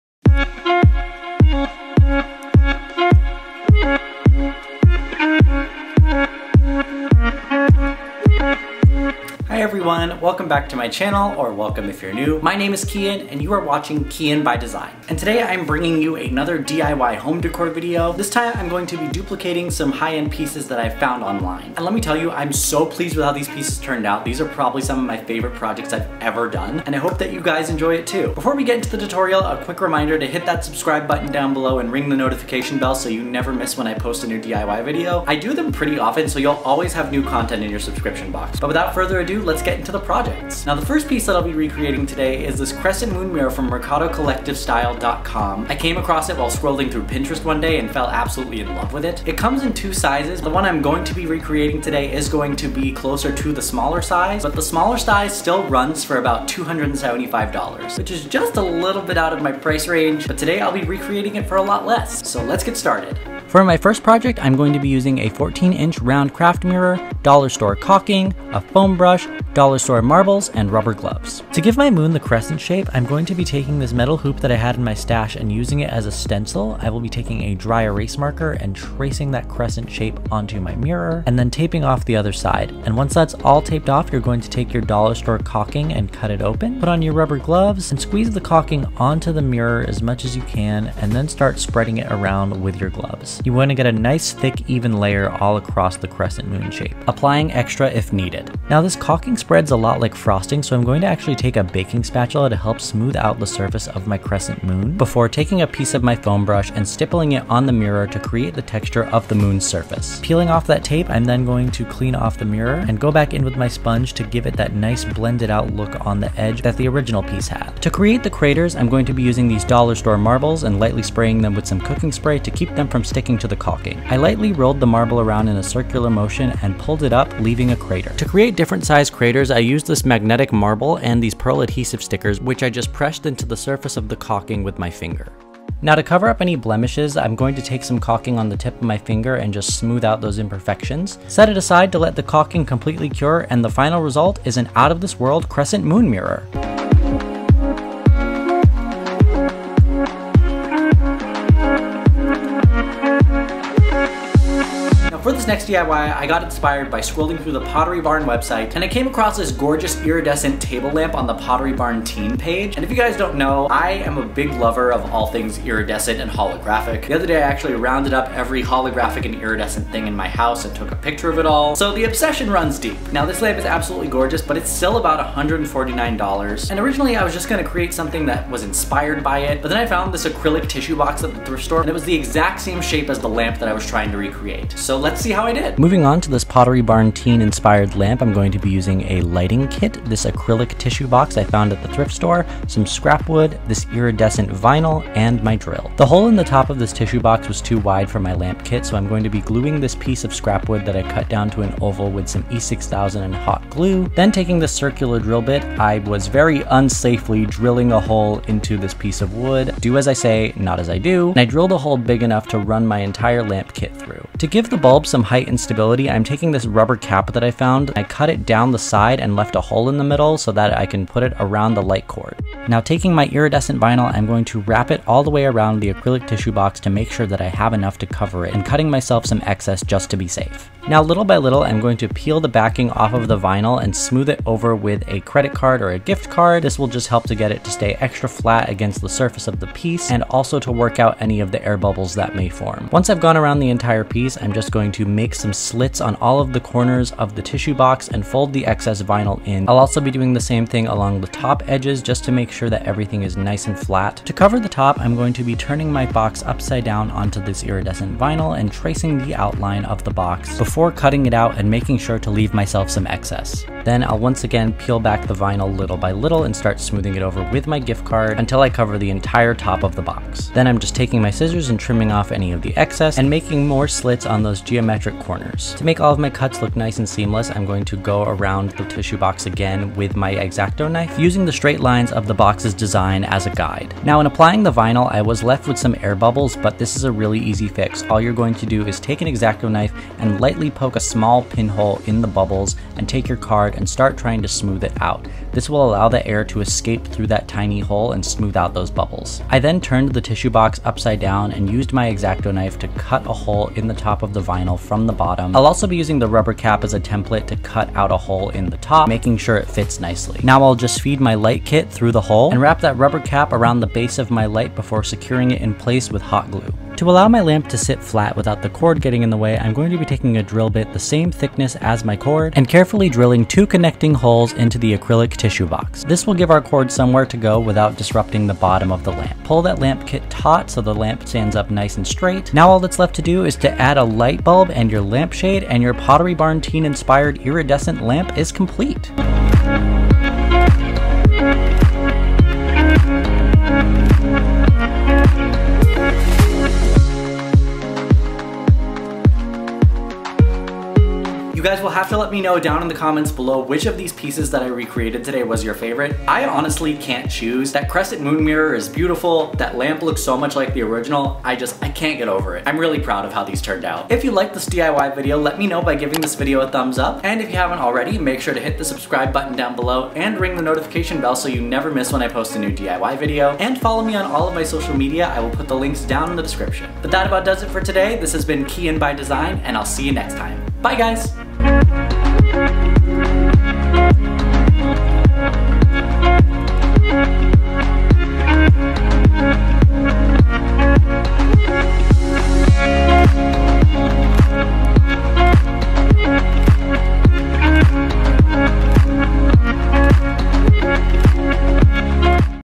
DIY Home Decor Dupes! DIY Moon Mirror & Pottery Barn Lamp Dupe! - Kyian By Design
DIY Home Decor Dupes! DIY Moon Mirror & Pottery Barn Lamp Dupe!

Wanna learn how I made these 2 ethereal DIY decor dupes? Watch this video for an easy how-to!